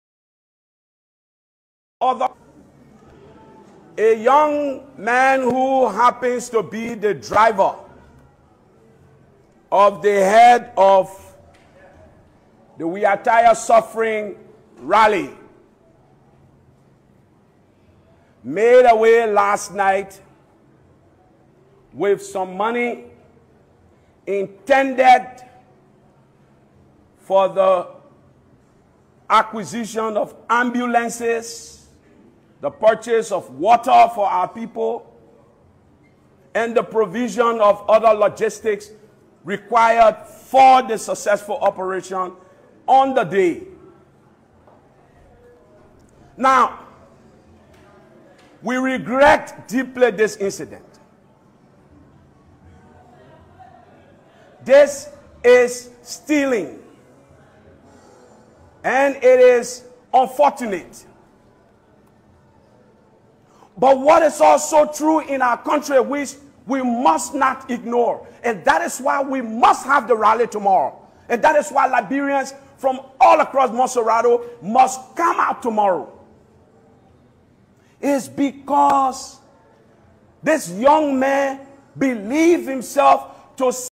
Driver Escapes with 70K Cash intended for Anti-Government protest
A driver assigned to an executive of the opposition CPP has escaped with 70K US dollars intended for Anti-Government protest to be held on December 17,2022 in Monrovia